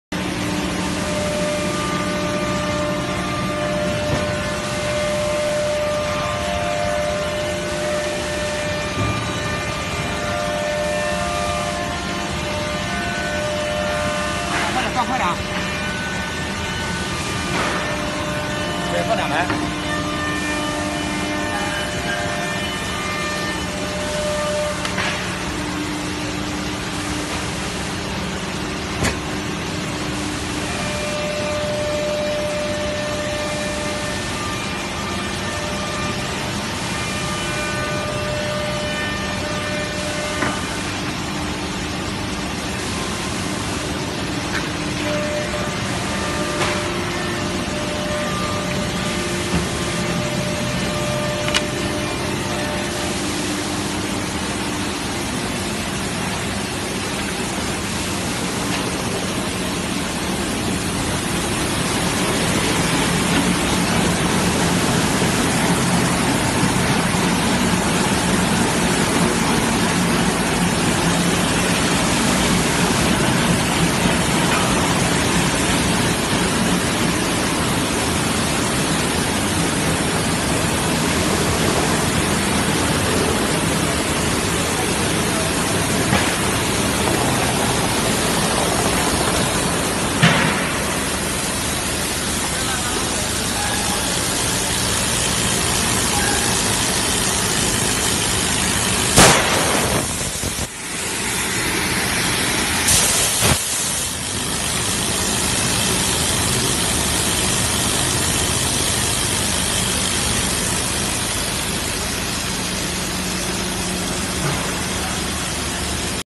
sanding machine/ profile sander
This machine is used for sanding the surface of panel , sanding strips can be effectively into the groove for sanding.
Both before paint and after primer paint can be sanded. It can save labor and improve efficiency.
It's our company's best seller.

Elly Chen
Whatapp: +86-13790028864
Wechat: MssElly